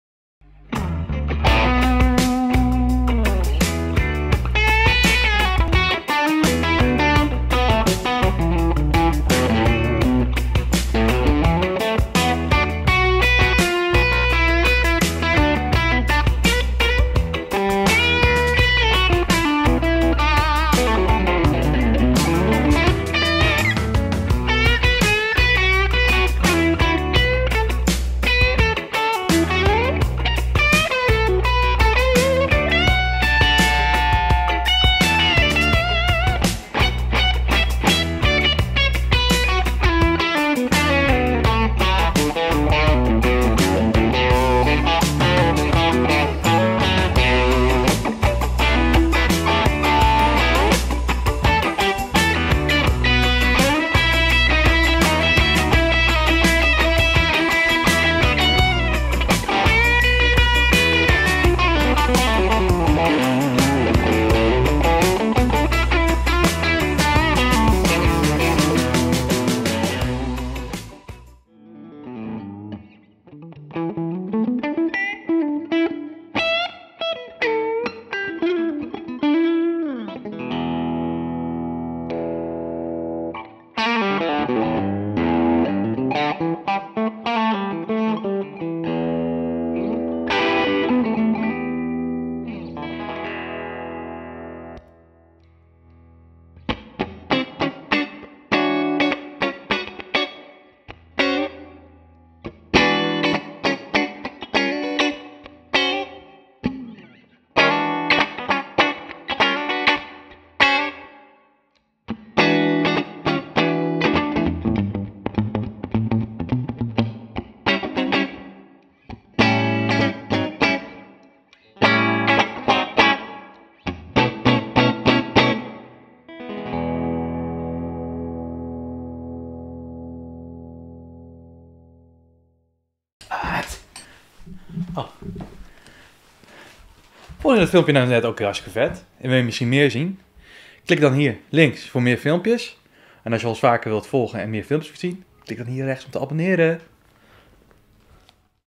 Maybach T61 Custom 3-TONE Sunburst Aged - Electric Guitar (SOUND DEMO)
Recorded with:

SE Electronics V7x
SE Electronics VR1
Focusrite Scarlett Audio Interface
Fabfilter L2 Limiter

TIJDCODES
00:00 - 01:10 With Music
01:10 - 02:10 Without Music